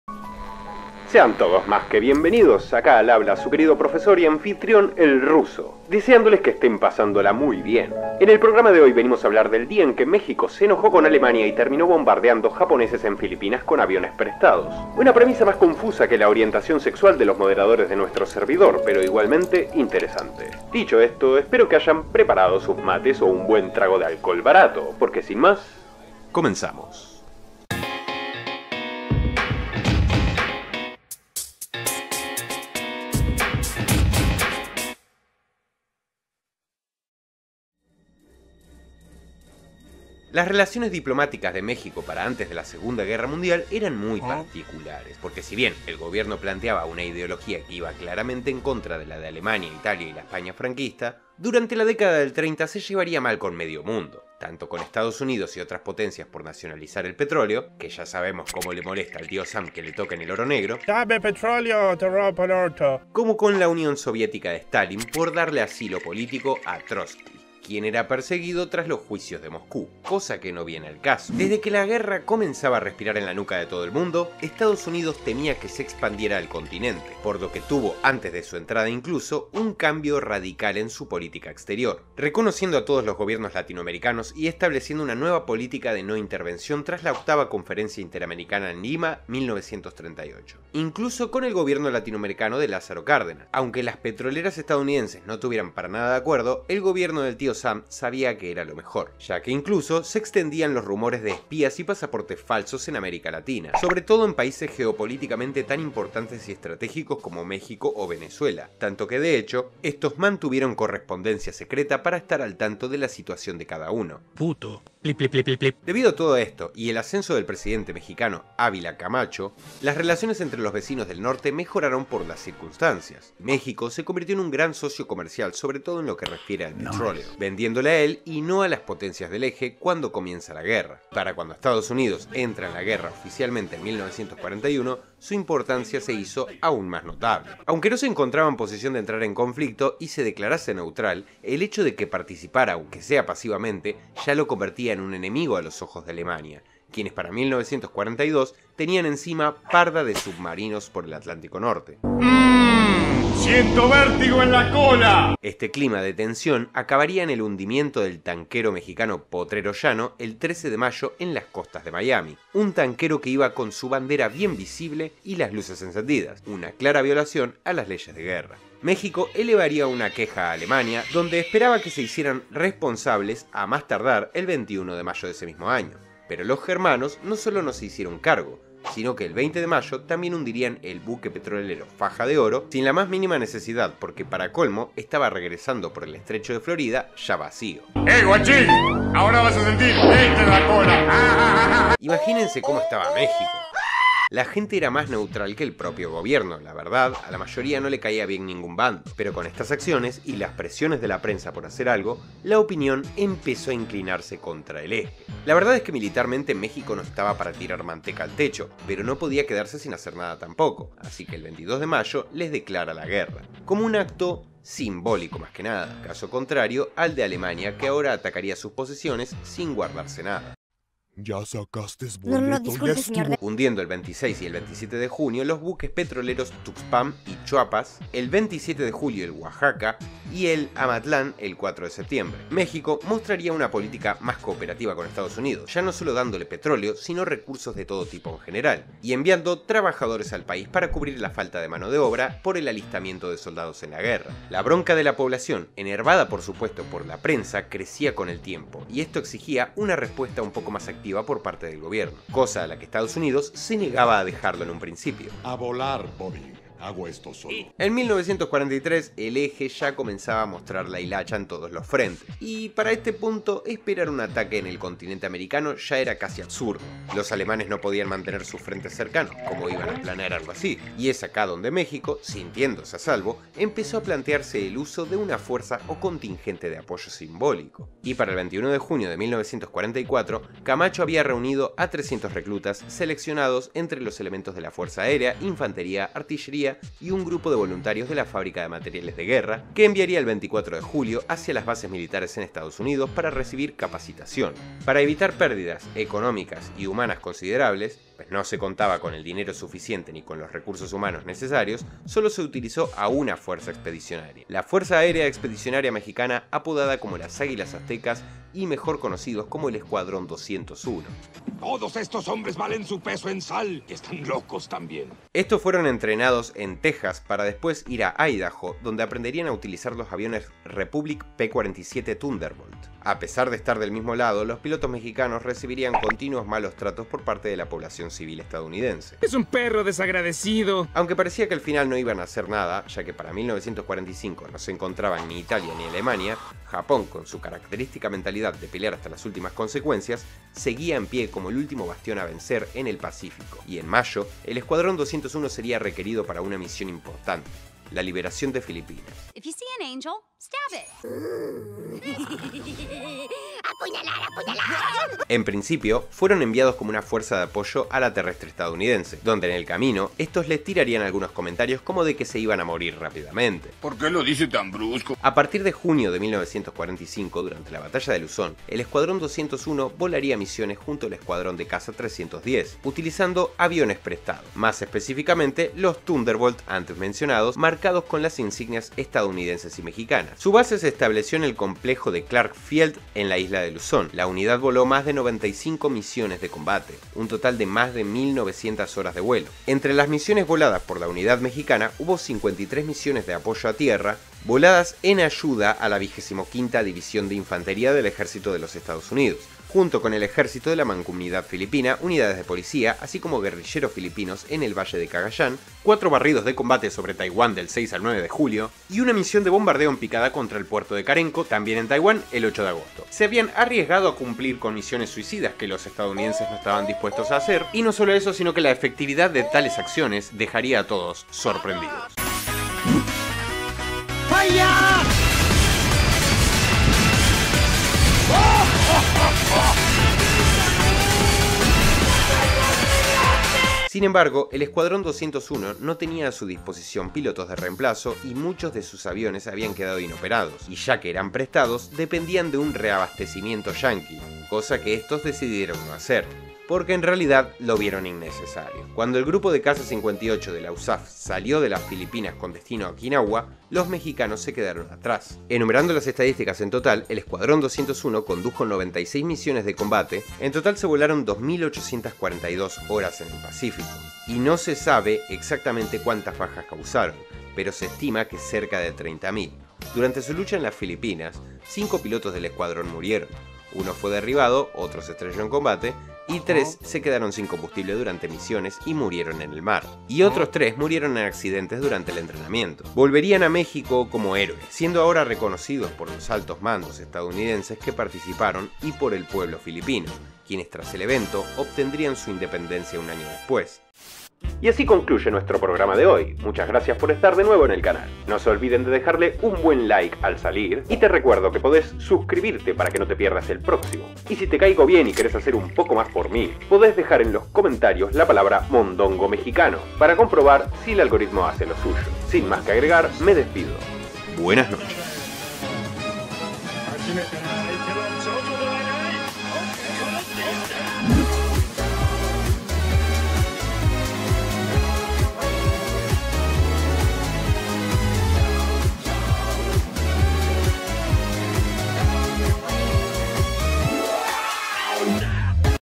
El dia que MEXICO ENFRENTO al IMPERIO JAPONES | SEGUNDA GUERRA MUNDIAL | HISTORIA con MEMES
En 1945, finales de la segunda guerra mundial, México enviaría a un grupo de hombres. El Escuadrón 201 mejor conocido como las Águilas Aztecas para enfrentar a los japoneses en Filipinas en apoyo a Estados Unidos.